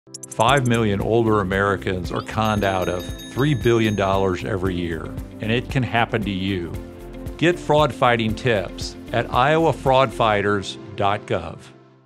Fight Fraud - Iowa Fraud Fighters
Every year, over 5 million older Americans are conned out of billions of dollars. Prevent this from happening to you and visit IowaFraudFighters.gov for resources on common investment scams.